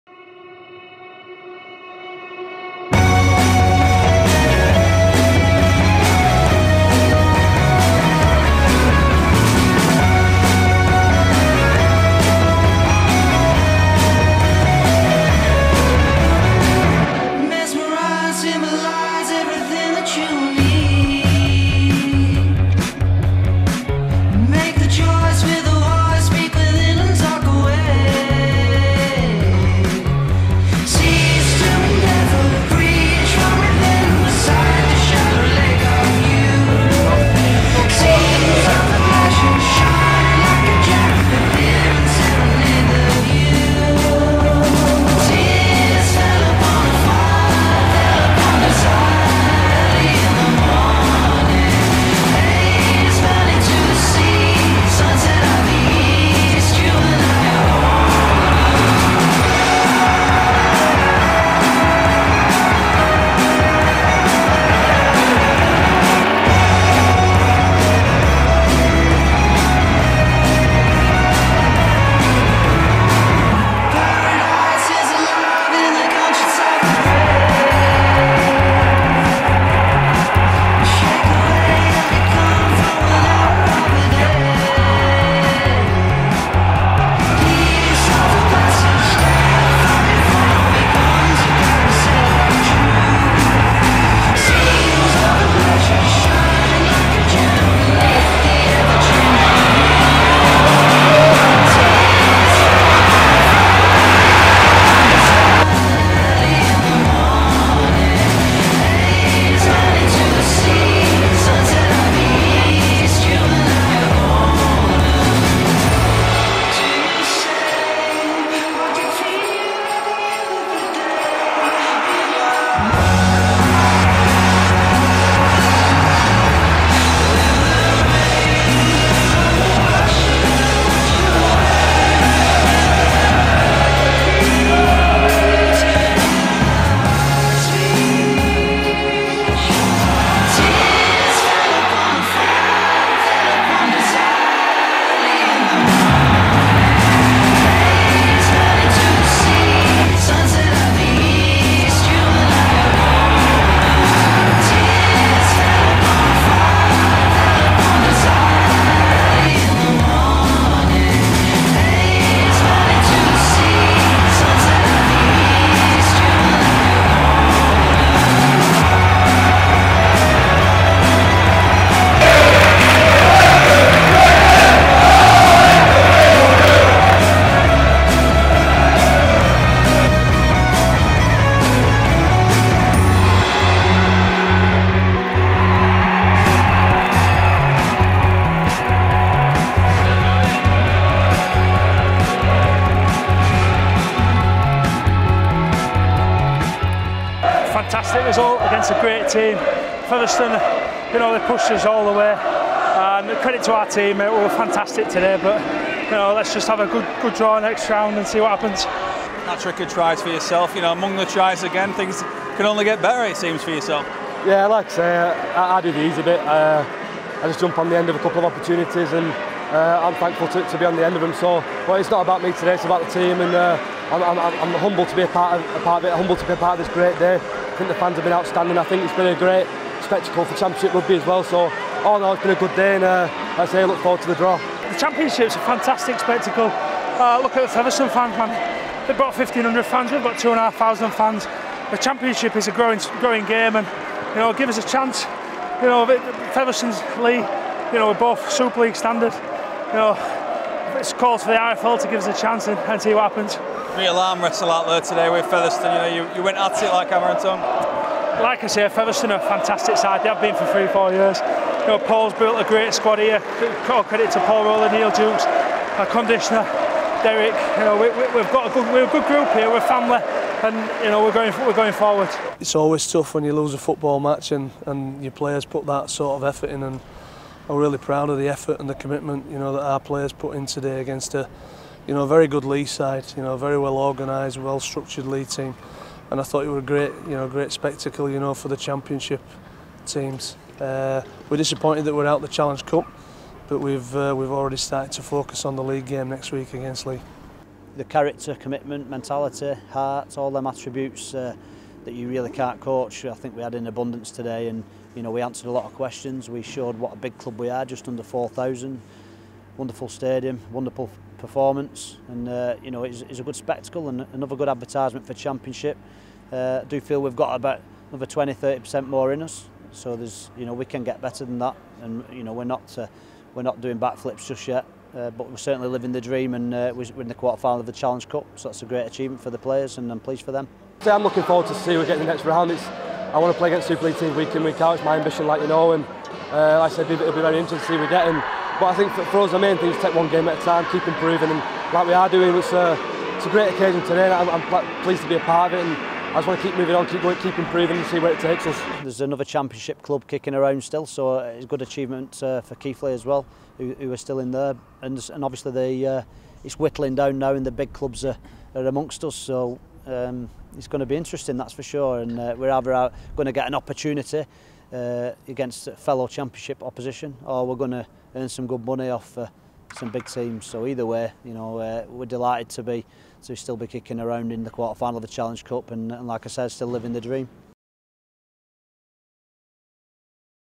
SLTV: Leigh keep the Cup dream alive
Leigh Centurions ran out 26-16 winners in the all Championship, Tetley's Challenge Cup tie against Featherstone Rovers. See the action and hear reaction from Sam Barlow, Ryan Brierley, John Bastian and Paul Rowley.